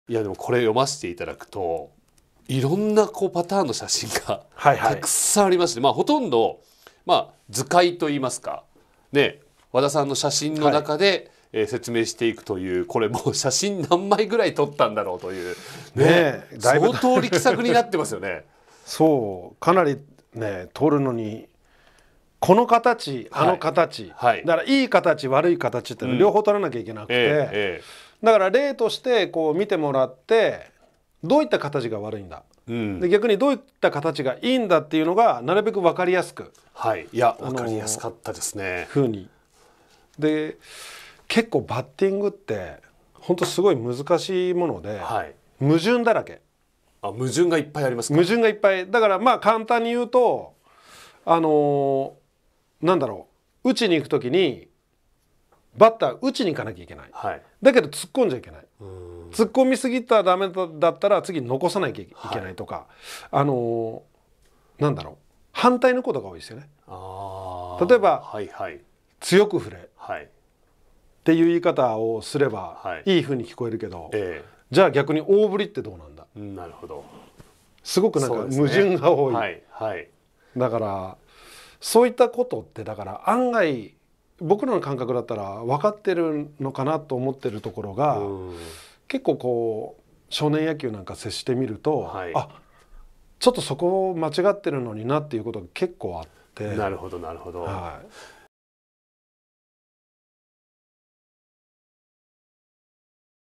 【書籍発売記念Special対談】元フジテレビアナ/田中大貴が聞く！中日ドラゴンズ/和田一浩が思う矛盾だらけの打撃理論とは！？
【書籍発売記念Special対談】和田一浩が思う最近の打者（和田一浩×田中大貴）

現役時代、西武ライオンズ、中日ドラゴンズで活躍し、通算2050本安打を記録し名球会入りも果たした右打ちの強打者「和田一浩」さんによる初実用書『ワンランク上を目指す 右バッターに伝えたい! 右打者のための究極打撃理論』の発売（3月24日から全国で順次発売）を記念して、元フジテレビアナウンサーでスポーツ番組の金字塔すぽるとで長年司会を務めた「田中大貴」さんとの対談がこの度実現!!書籍の魅力はもちろん、二人の野球熱、野球知識が溢れ出る至極の対談をぜひお楽しみください!!

▶書籍はコチラ